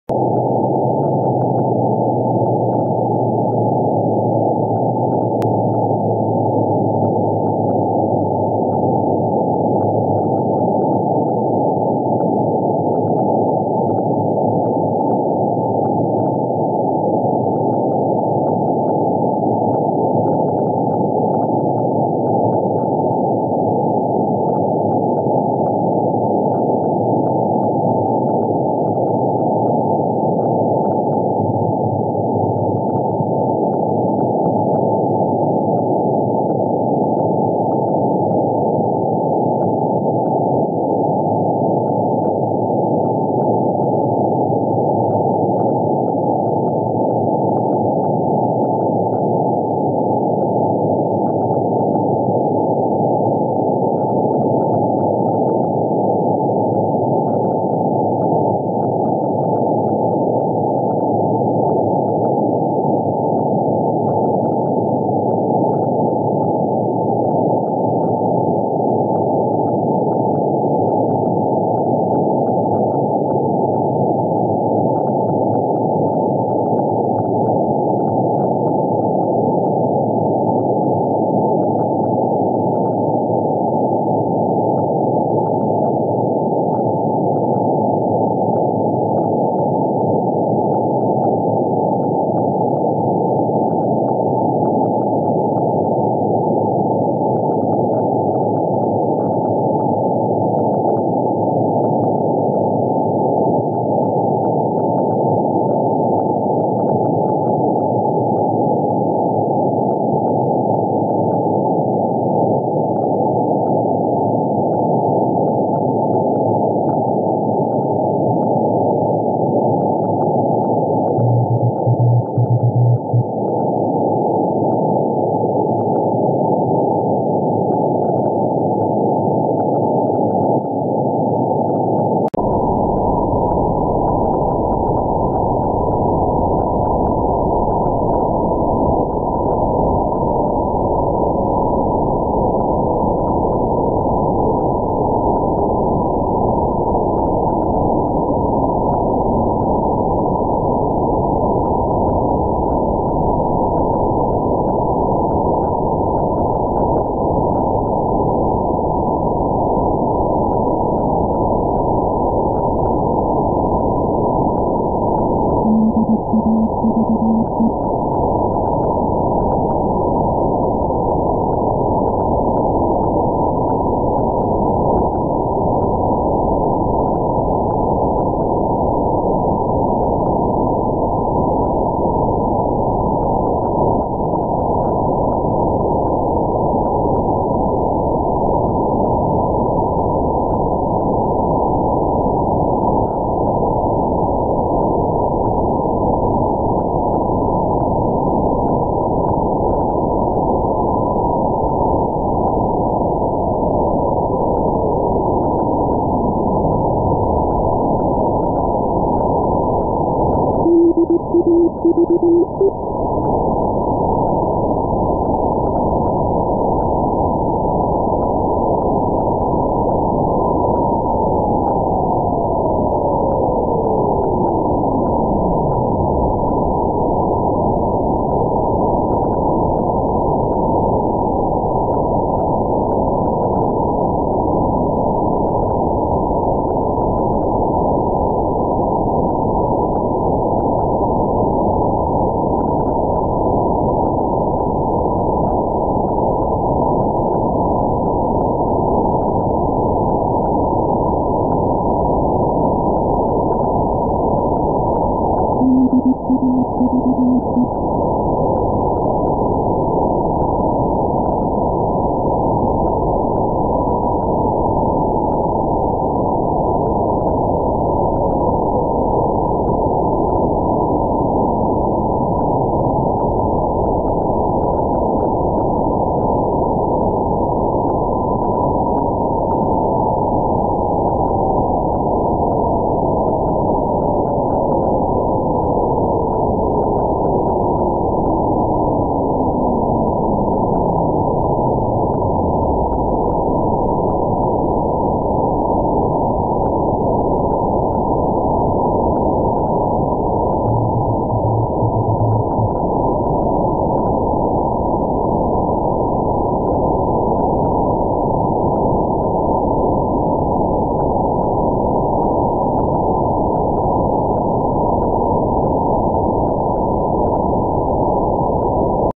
2018-10-05: CP-7 (DAVE): CW @ 437.15 MHz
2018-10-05: CW signal from CP-7 (DAVE) at 437.15 MHz with RTL SDR

Signals from ELFIN-A and ELFIN-B can also be seen (437.475 MHz) during the pass.